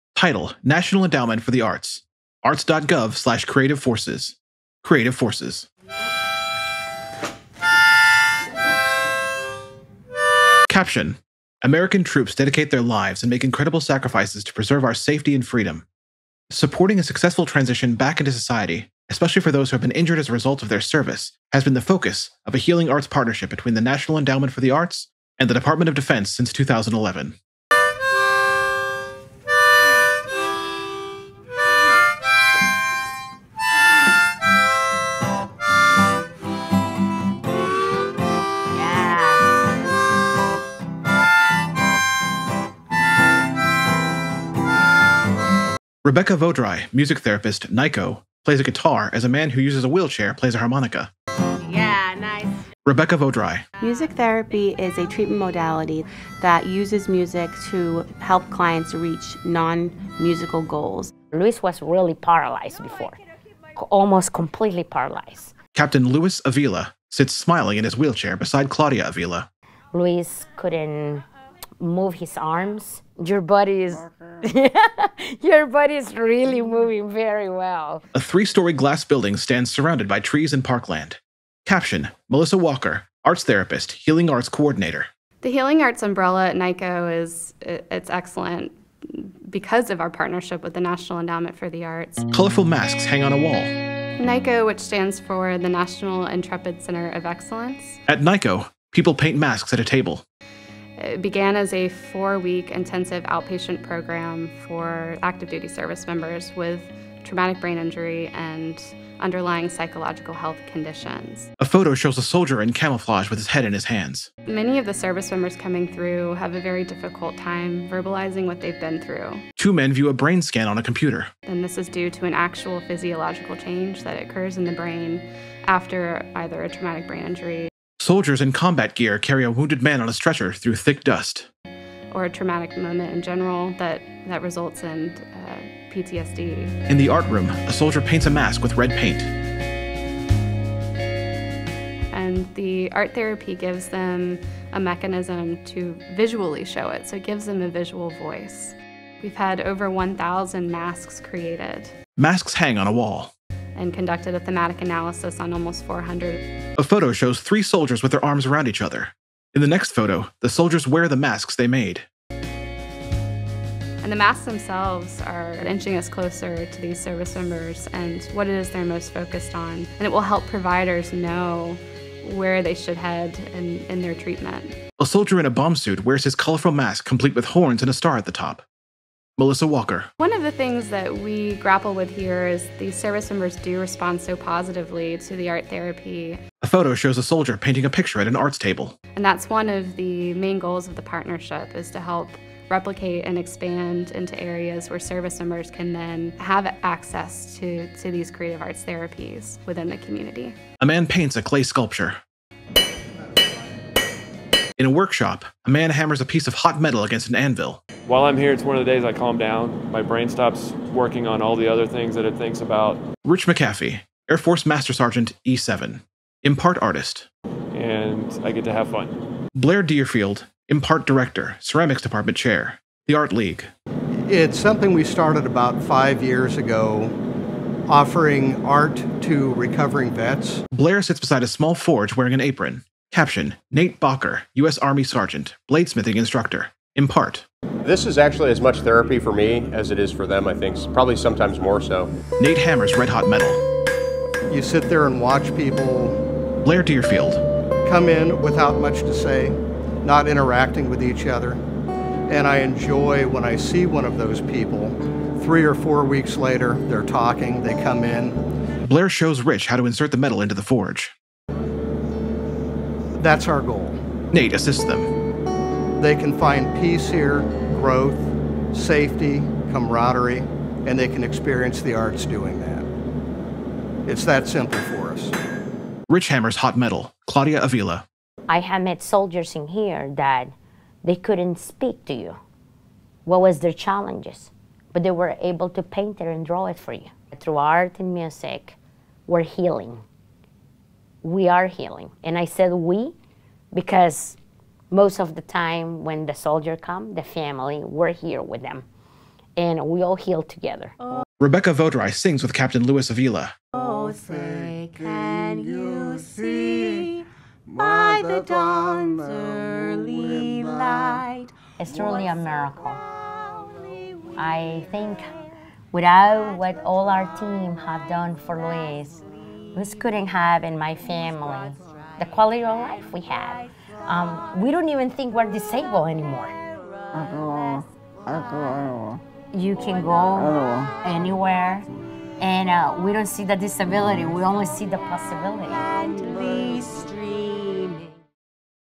Creative Forces: NEA Military Healing Arts Network (Audio Described Version)
Creative Forces®: NEA Military Healing Arts Network seeks to improve the health, well-being, and quality of life of trauma-exposed military service members and veterans, as well as their families and caregivers, by increasing knowledge of and access to clinical creative arts therapies and community arts engagement.

The program places creative arts therapies at the core of patient-centered care at clinical sites throughout the country, including telehealth programming, and increases access to community arts activities to promote health, wellness and quality of life for military service members, veterans, and their families and caregivers.